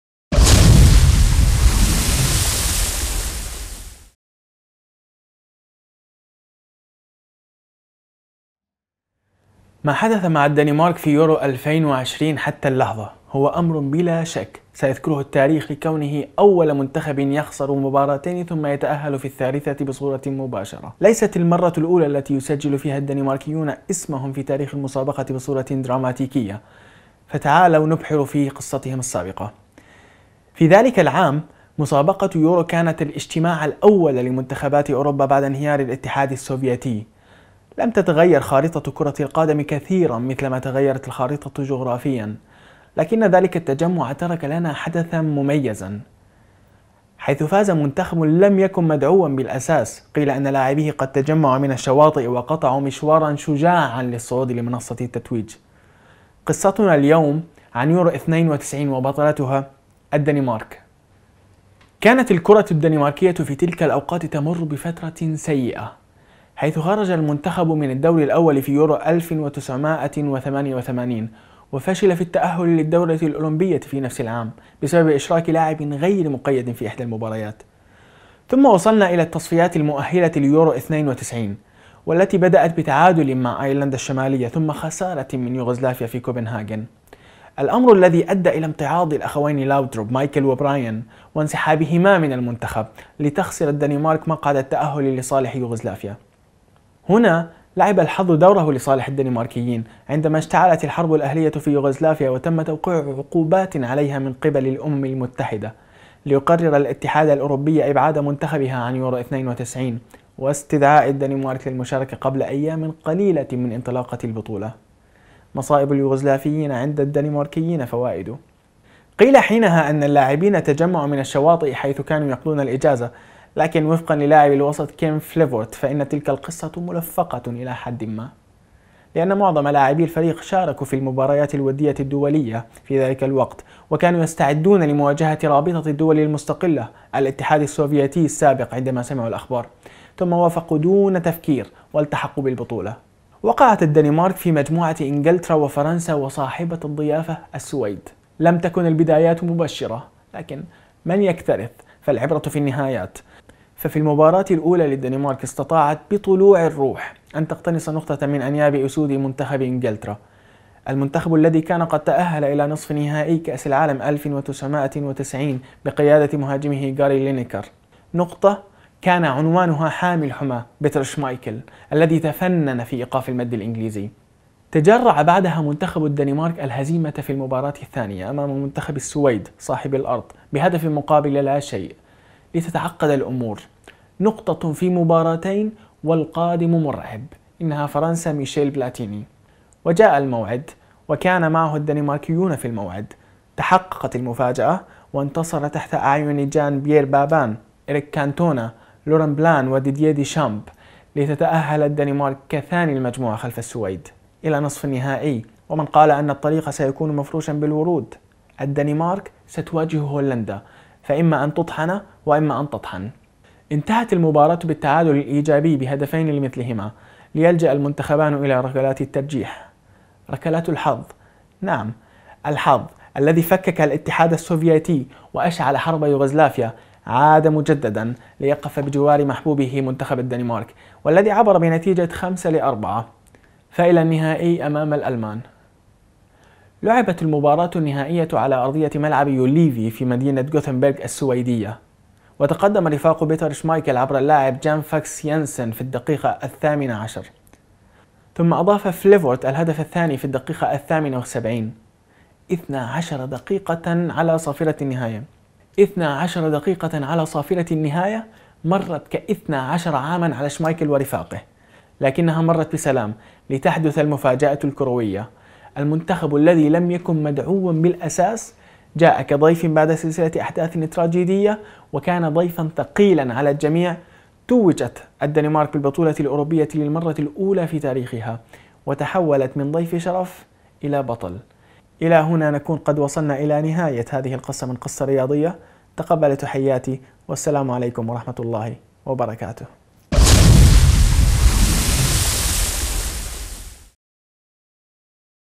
الدنمارك .. المنتخب الأكثر إثارة في تاريخ أمم أوروبا
قصة الدنمارك .. المنتخب المثير في اليورو.

حساباتي في شبكات التواصل:
تيلغرام :